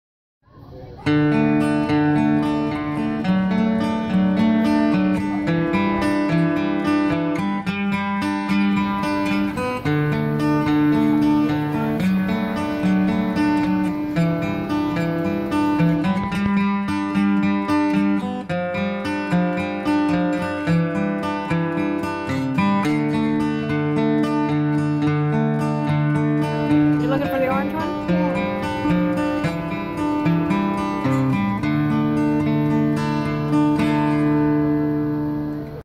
Experiencing the Spring Butterflies at the Frederik Meijer Gardens | Grand Rapids, Michigan
Experiencing the butterflies at the Frederik Meijer Gardens in Grand Rapids, Michigan.